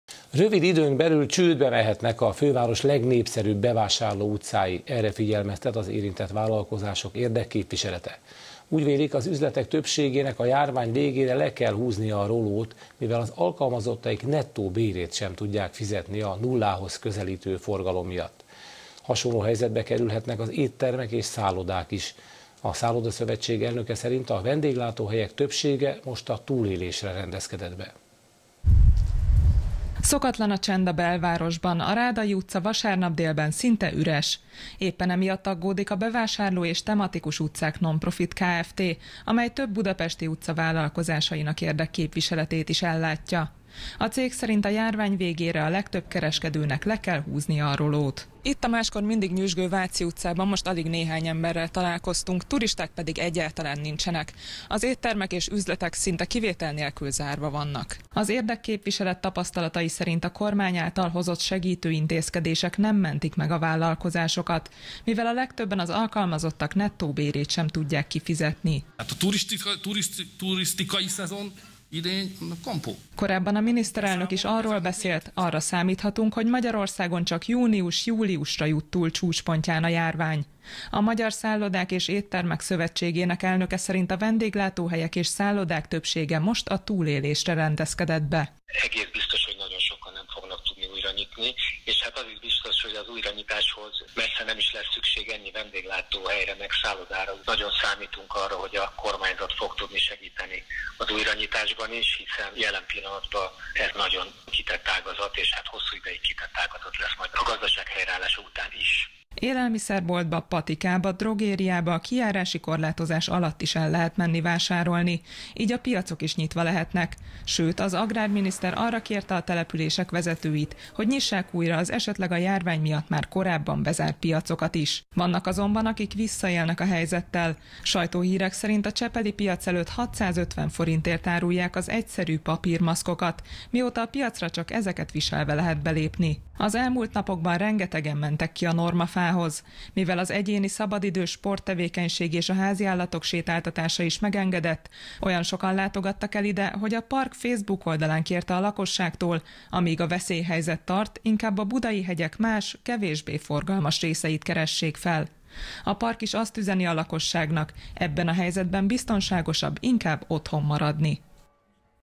Csődbe mehetnek a főváros legnépszerűbb bevásárlóutcái
Rövid időn belül csődbe mehetnek a főváros legnépszerűbb bevásárlóutcái – erre figyelmeztet az érintett vállalkozások érdekképviselete. Úgy vélik, az üzletek többségének a járvány végére le kell húznia a rolót, mivel az alkalmazottaik nettó bérét sem tudják fizetni a nullához közelítő forgalom miatt.